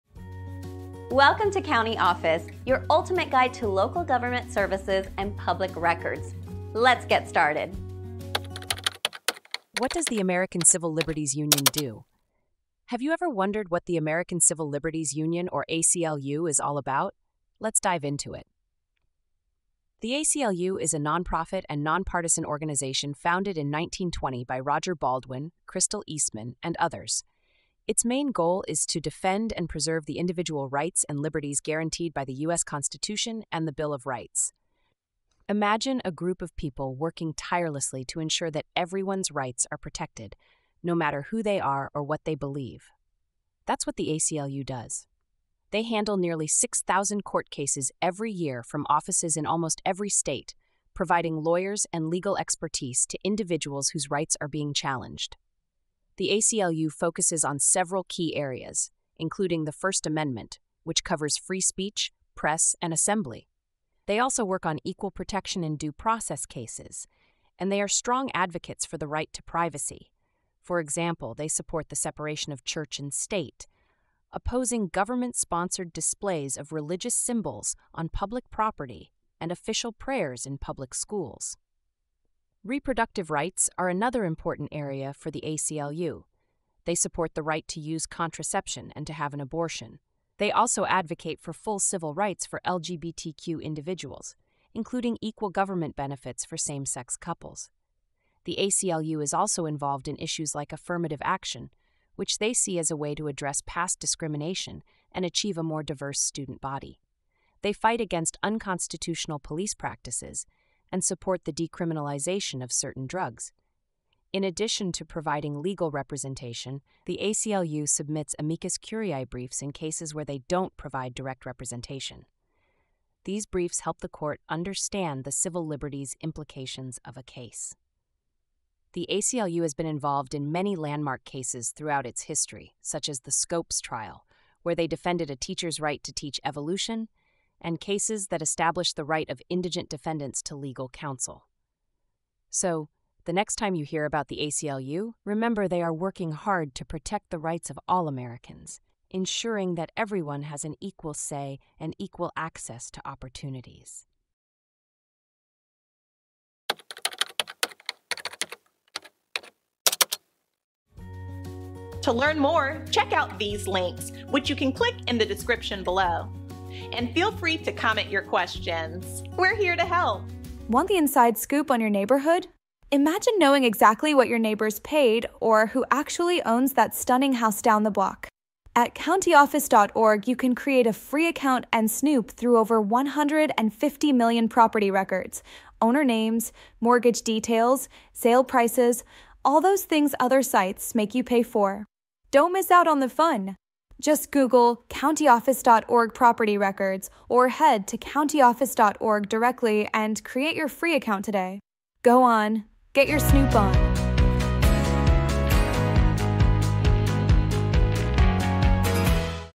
What Does the American Civil Liberties Union Do? - CountyOffice.org
What Does the American Civil Liberties Union Do? The American Civil Liberties Union (ACLU) plays a vital role in protecting the rights and freedoms guaranteed by the U.S. Constitution. In this enlightening video, we will explore the mission and activities of the ACLU, a nonprofit and nonpartisan organization founded in 1920. We will discuss how the ACLU defends individual rights through legal representation and advocacy, handling thousands of court cases each year across the nation.

The ACLU's work encompasses various critical areas, including First Amendment rights, reproductive rights, and equal protection under the law. We will highlight their efforts to maintain the separation of church and state, support for LGBTQ rights, and their stance on affirmative action. Additionally, we will touch upon their involvement in significant legal cases that have shaped civil liberties in America, illustrating the essential role they play in our society.

For anyone interested in understanding how civil liberties impact their lives and the broader implications of the ACLU's work, this video is a must-watch. Join us as we delve into the ACLU's history, its landmark cases, and the ongoing challenges in the fight for justice and equality.